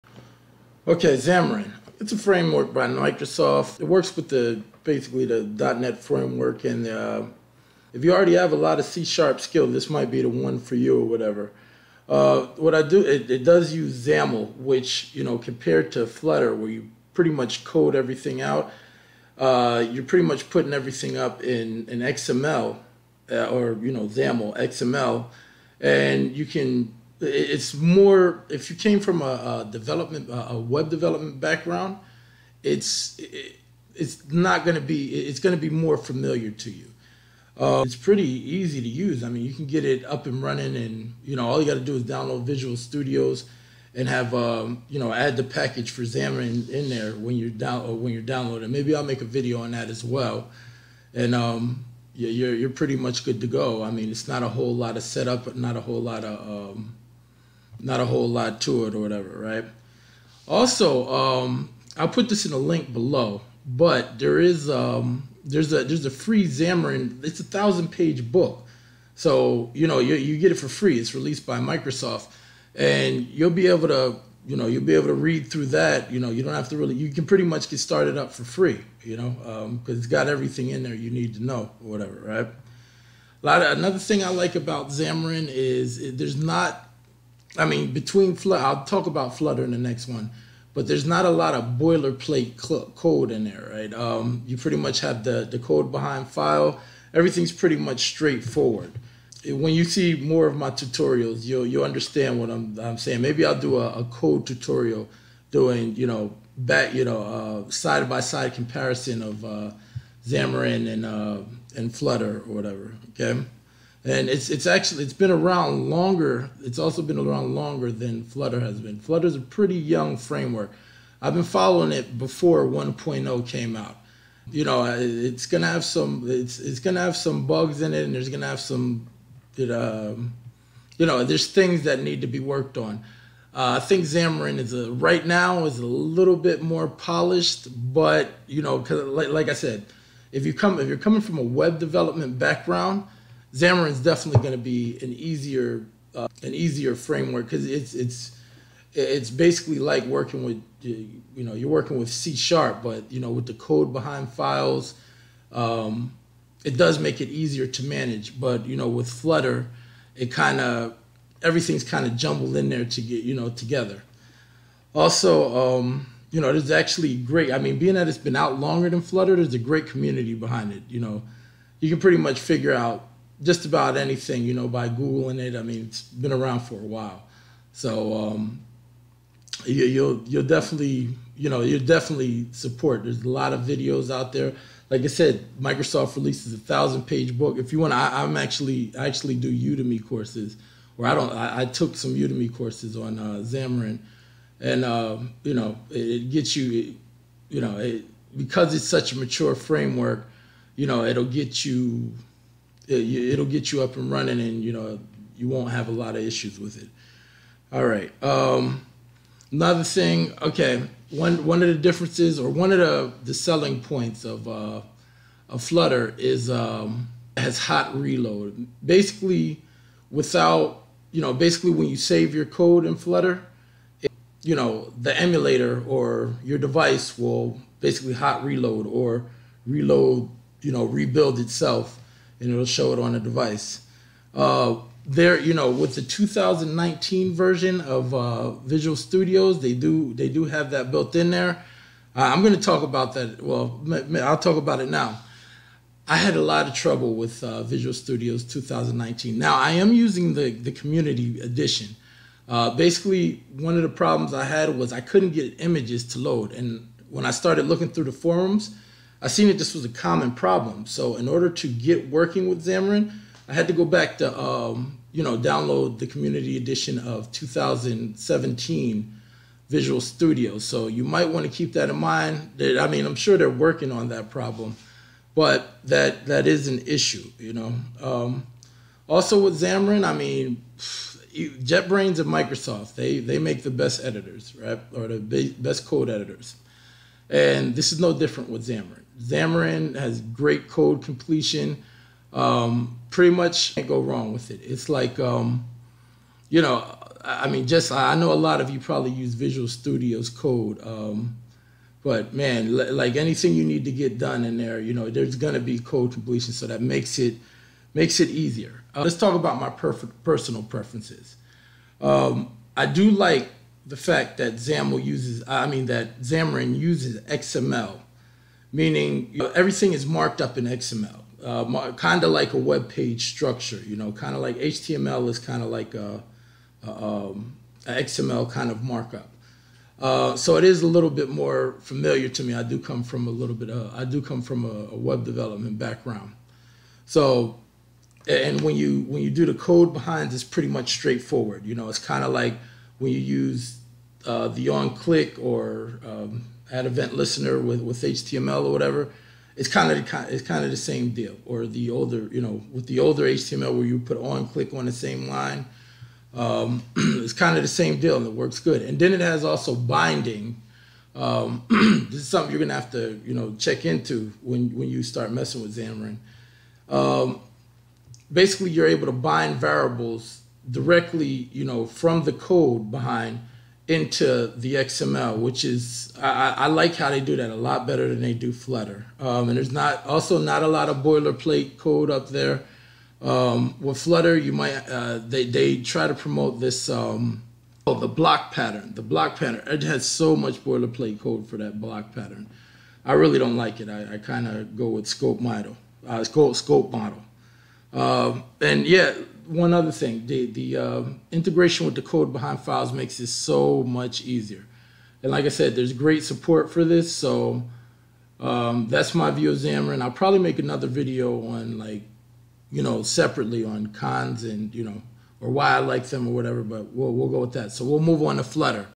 xamarin vs flutter part 1
Comparison between flutter and xamarin. Two frameworks for creating mobile apps